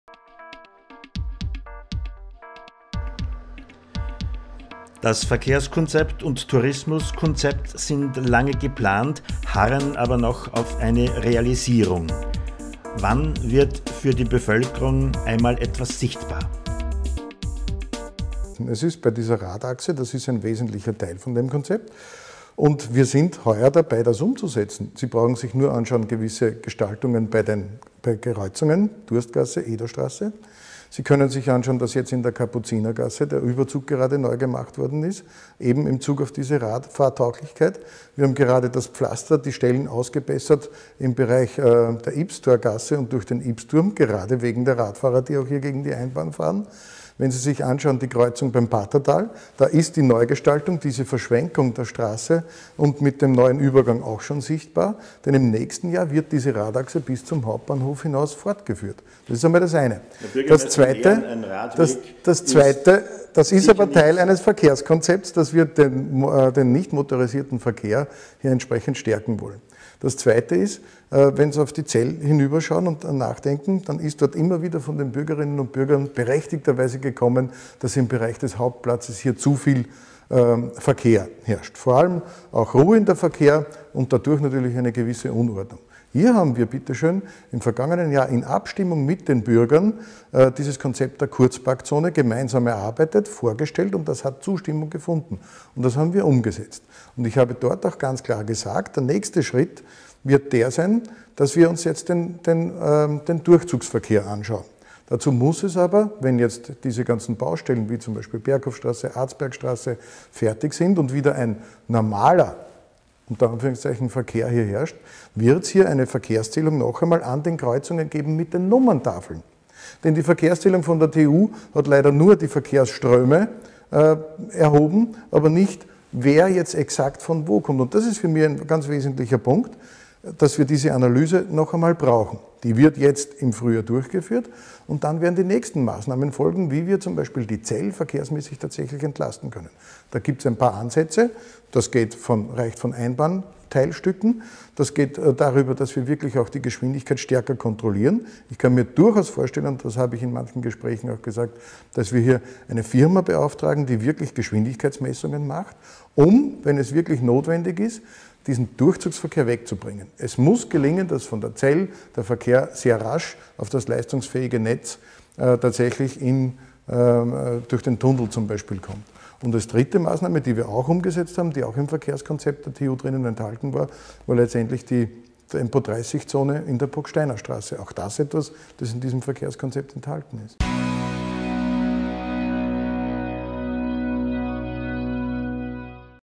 Teil 5 Waidhofner Bgm. Mag. Werner Krammer im News-Gespräch
Der Bürgermeister Talk. YBBSTALNEWS.at führte mit dem vielbeschäftigten Bürgermeister der Ybbstalstadt ein Update Interview nach einem Jahr Amtszeit.